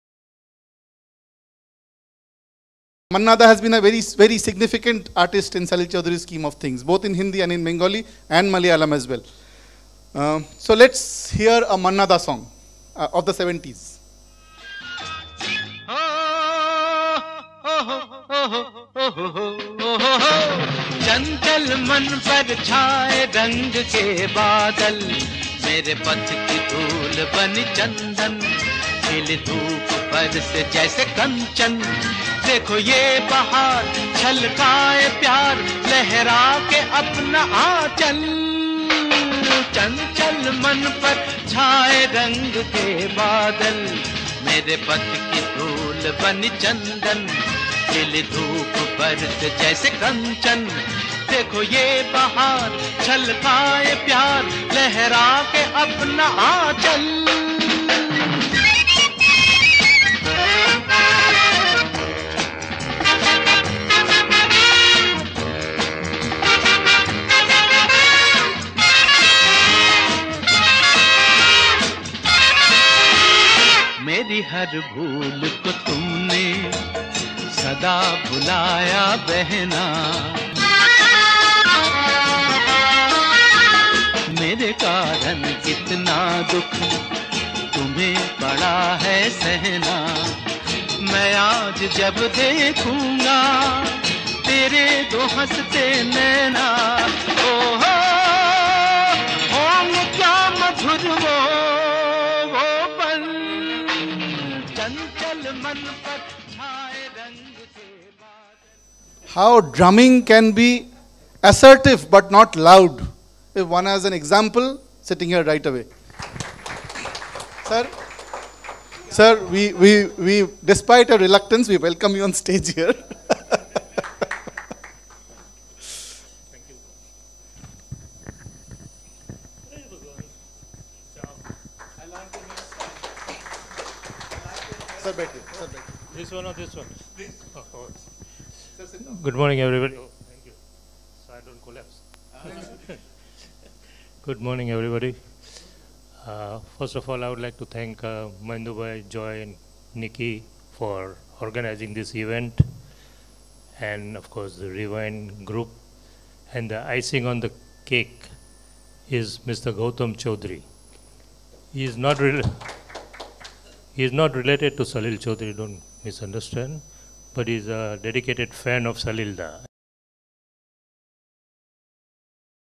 REWIND - NED (Never Ending Discovery) Workshop on Salil Chowdhury in Surat - Jan 2018
This clip is from the NED (Never Ending Discovery) show on Salilda presented by Team REWIND for an exclusive audience in Surat on 21st Jan 2018

In this clip Archisman Team REWIND sets the mood  for the introduction of master drummer Bujor Lord by playing a song in which he has played the drums with aplomb.

Hear Bujor Lord also speak on the stage at the event